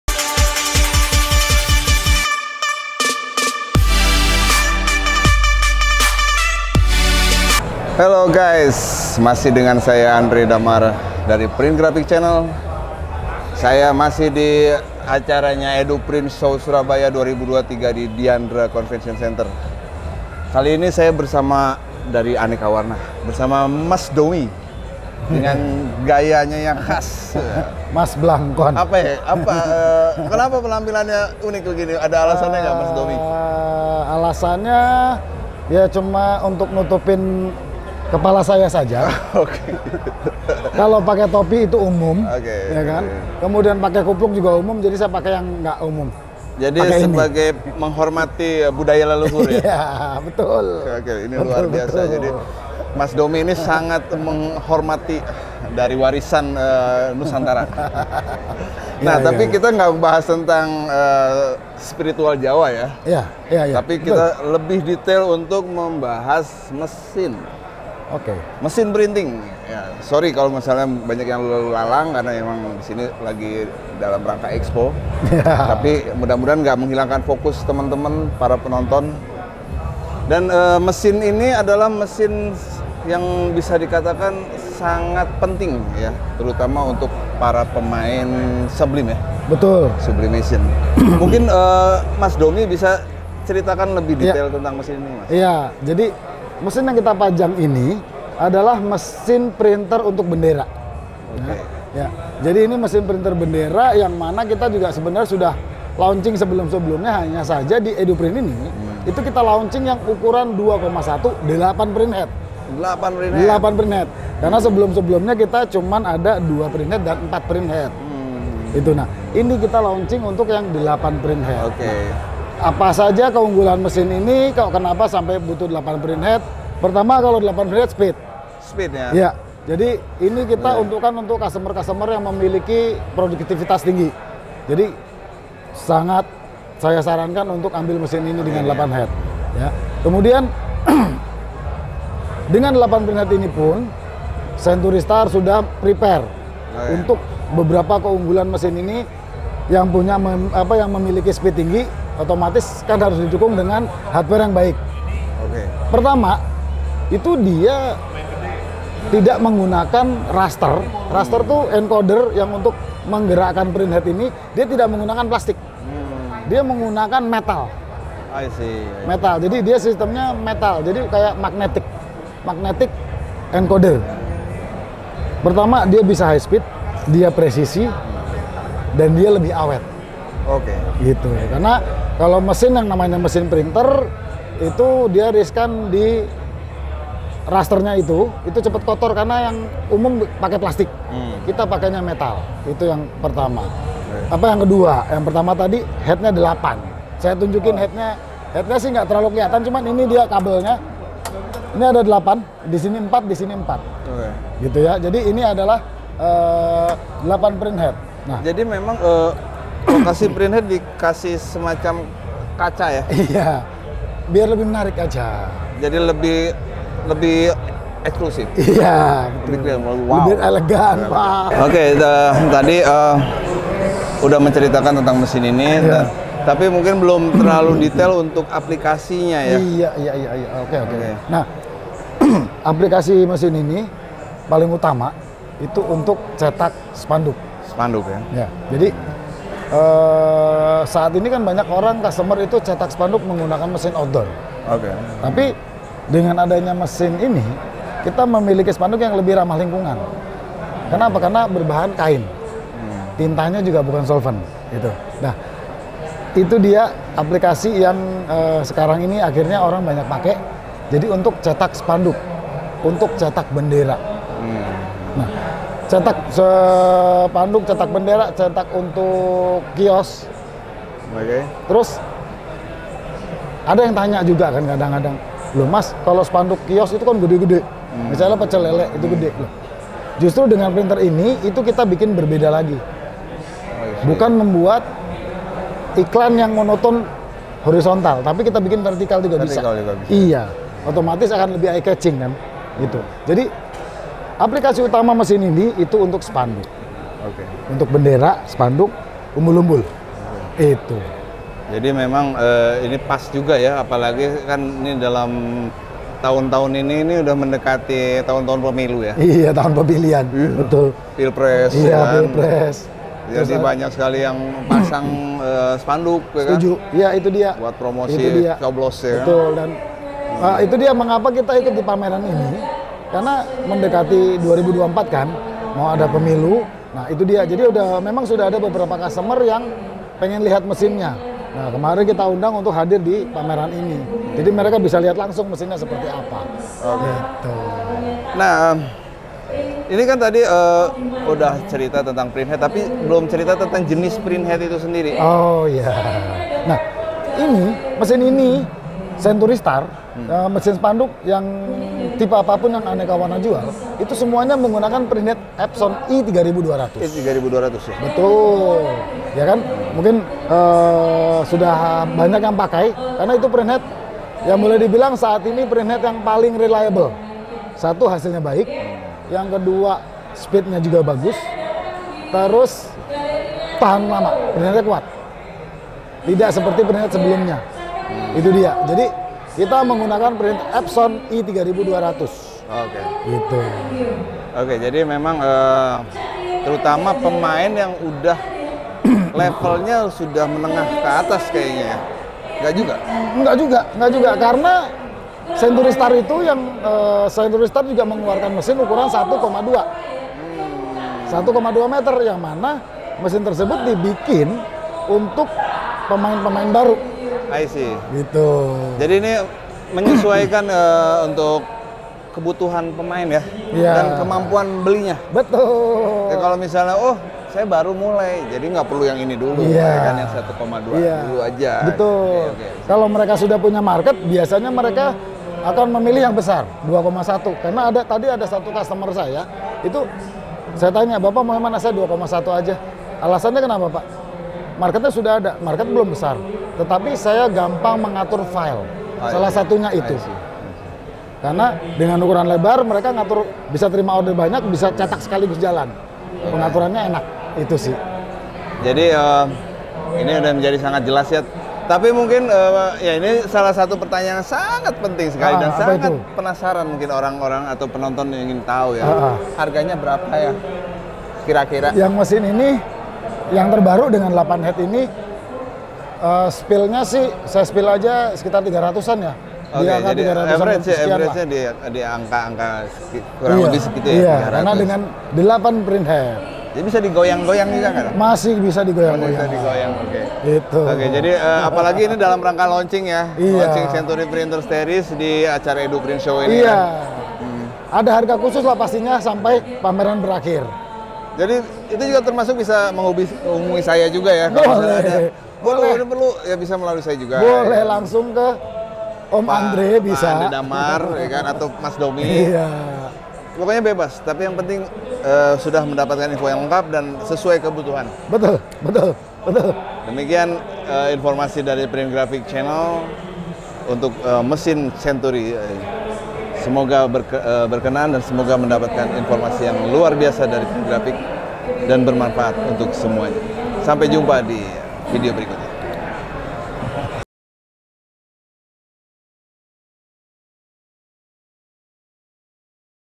Product Review Century Star CSP-2100 Dye Sublimation Printer dari Aneka Warna
06 Juni 2023 – Di Eduprint Show Surabaya 2023, PT Aneka Warna meluncurkan mesin digital printer Direct Dye Sublimation Ink  Century Star series. Ini merupakan mesin cetak direct to fabric flag and cloth banner. Yang terbaru adalah CSP-2100 dimana terdapat pilihan unit dengan 4 set printhead dan unit dengan 8 set printhead.

Century Star CSP-2100 memiliki kecepatan hingga 210 m2 / jam. Mesin unggulan ini menggunakan printhead Epson i3200.

Hardian Lorens (Owner PT Aneka Warna) mengatakan kepada Redaksi bahwa penggunaan mesin untuk cetak direct to fabric ini lebih ramah lingkungan (go green) karena berbasis air (water base) dan banyak aplikasi yang menggunakan material kain dengan grammmatur 100 gsm ke bawah, dibanding flexy dengan grammature yang lebih berat (250 gsm). Ini menjadi alternatif bagi para pegiat digital printing, apalagi bukan hanya aplikasi pembuatan spanduk saja, tapi juga flag, dekorasi pesta, tenda, kanopi, hanging banner, separator banner, Parimeter banner, dan sebagainya.

Century Star merupakan salah satu market leader penyuplai material dan mesin digital printing di China. Dan kerjasama ini sudah cukup lama terjalin.

Info lebih lengkap untuk mesin Century Star CSP-2100 ini bisa menghubungi :
Print Graphic Channel di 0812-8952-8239